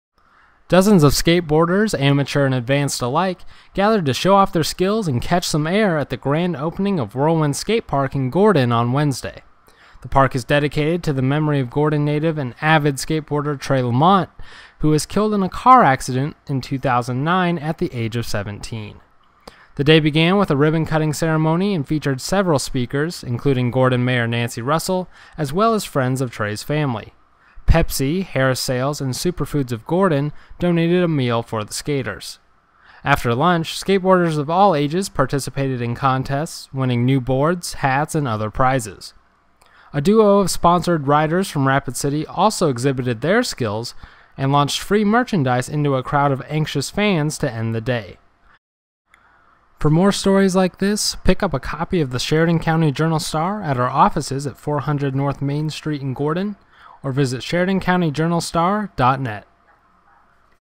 Whirl Wind Skate Park Dedicated in Gordon
A dedication was held for the new Whirl Wind Skate Park in Gordon, Nebraska on Wednesday, July 2, with free food, a skate competition and demo, and a blessing of the park.